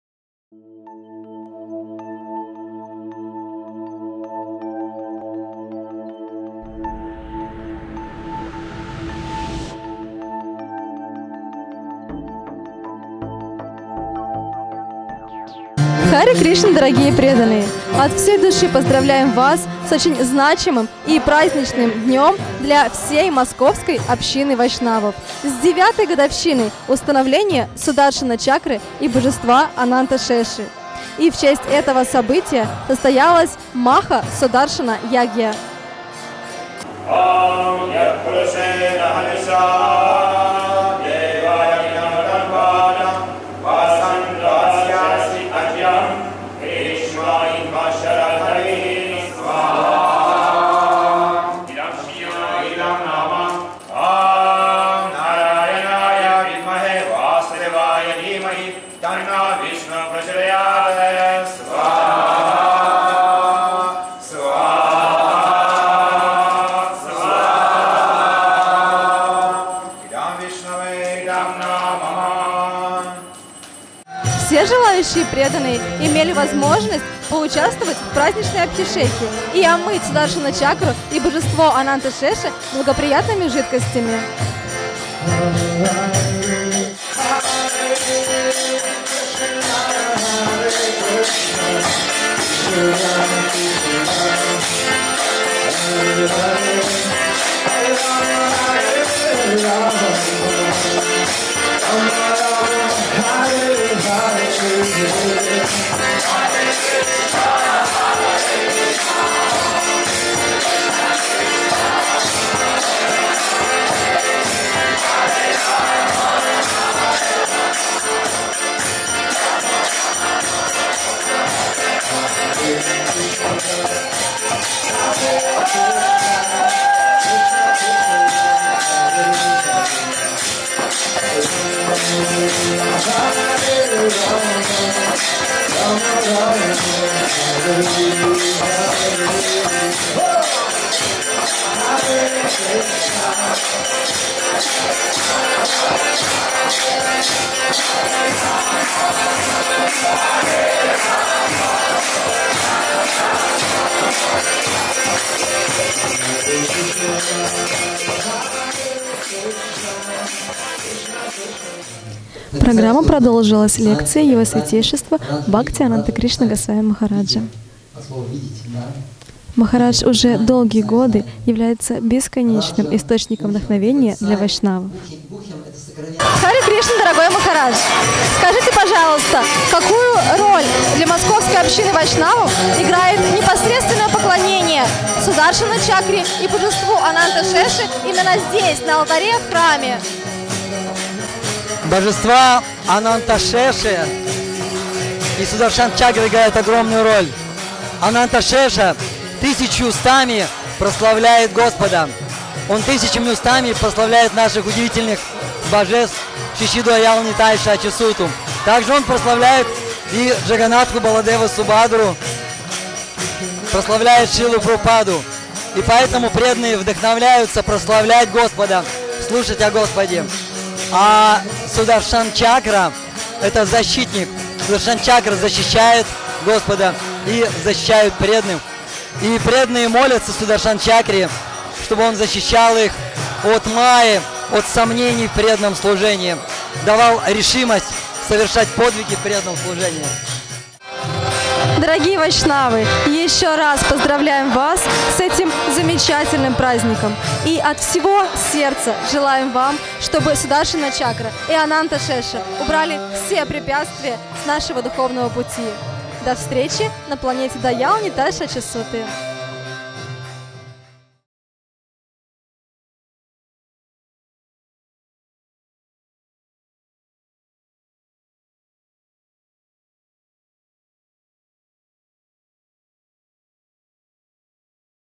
Праздник Сударшана-чакры и Божества Ананта-шеши.
2 Июля 2017 года в московском храме Шри Шри Даял-Нитай Шачисуты состоялось  празднование 9-ти летия установления Сударшана-чакры и Божества Ананта-шеши.